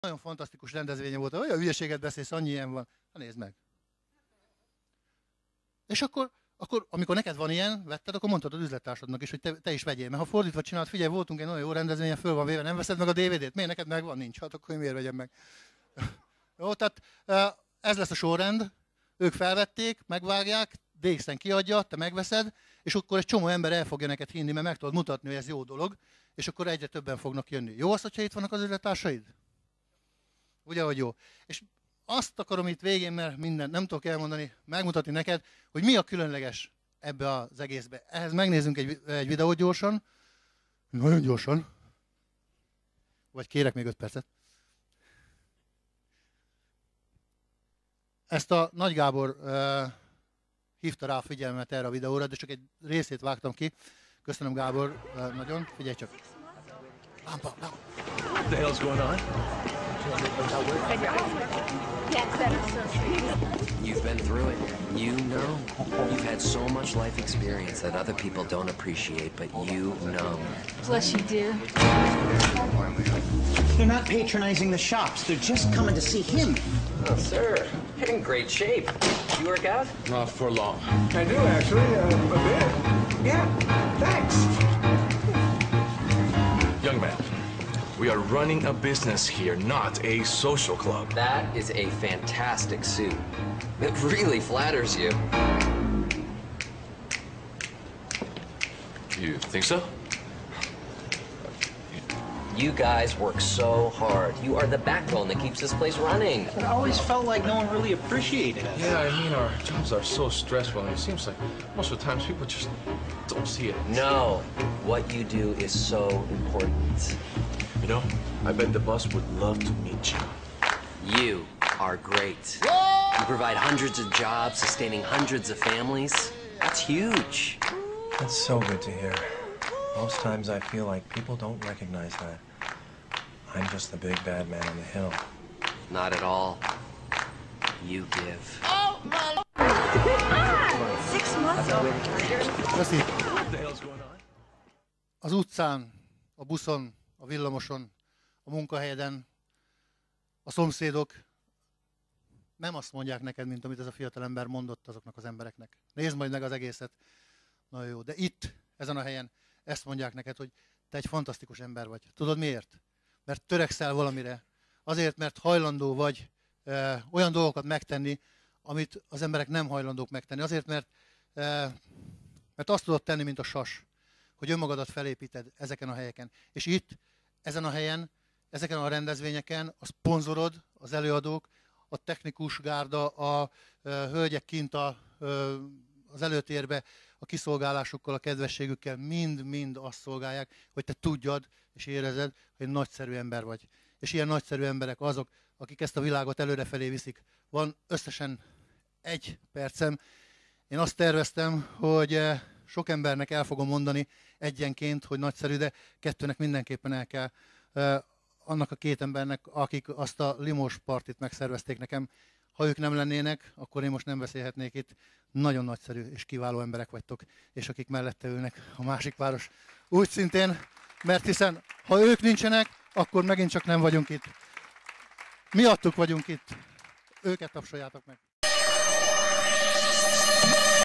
DXN Üzletépítő Szeminárium részlet 2011-ből
Előkerült ez a felvétel.
DXN rendezvényen, ilyen nagyon viszonylag ritkán kapunk szót, de abban bízom, hogy a ritka alkalmakon tudunk adni valamit a hallgatóságnak.
Ez egy rövid részlet.